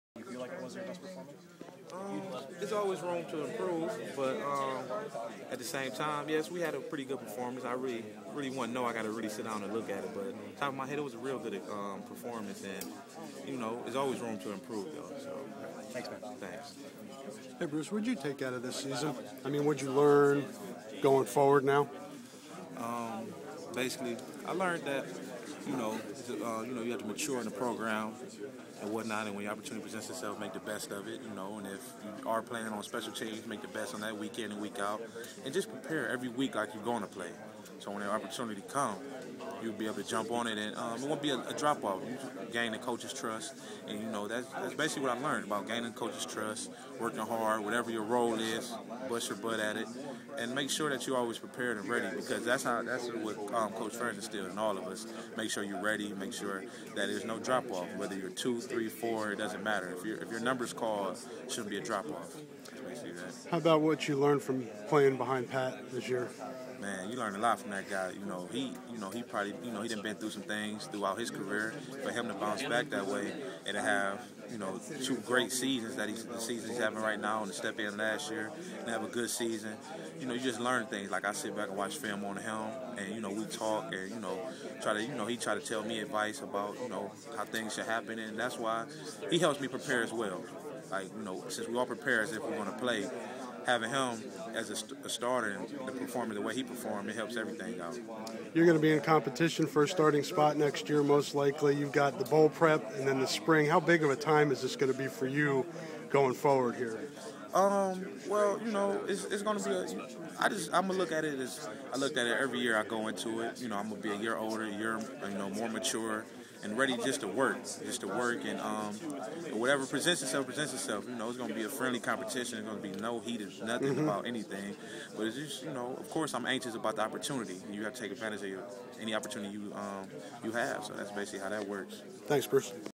Bruce Davis
Iowa sophomore talks about getting some reps on Saturday and looks ahead to the future.